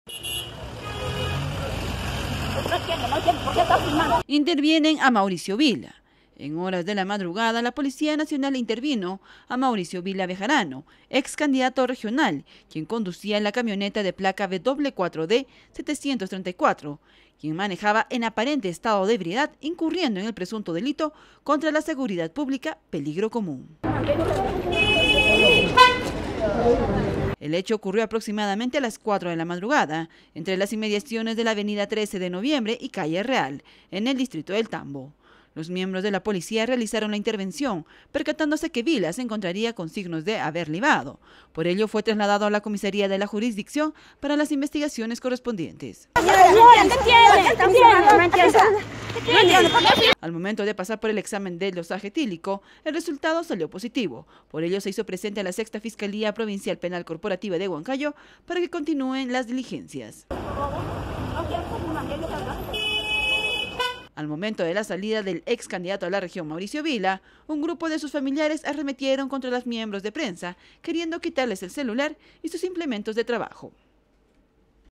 Huancayo: Intervienen a Mauricio Vila por peligro común
Intervienen a Mauricio Vila. En horas de la madrugada la policía nacional intervinieron a MAURICIO VILA BEJARANO, ex candidato regional, quien conducía la camioneta de placa W4D-734, en aparente estado de ebriedad, incurriendo en el presunto delito contra la seguridad pública -  peligro común.